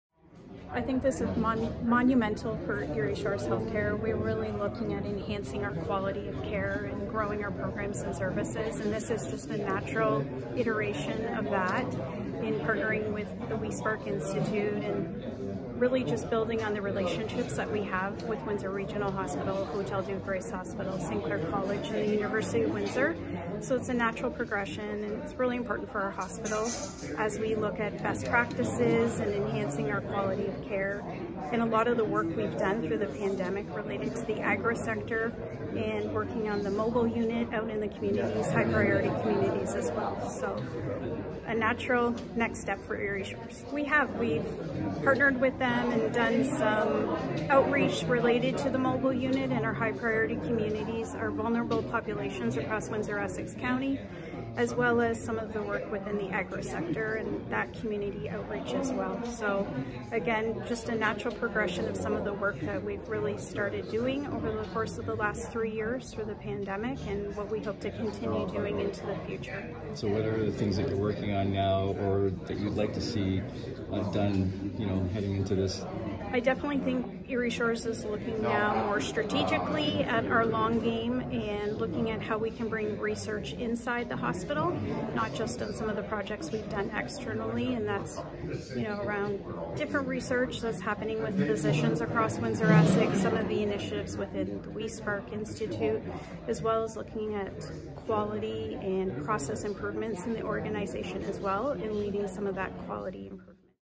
Erie Shores Healthcare Officially Becomes A Parnter Of We-spark Health Institute
Kristin Kennedy, president and CEO, Erie Shores HealthCare, talks about the importance of becoming an official partner of WE-SPARK Health Institute, Wednesday, Nov. 16, 2022.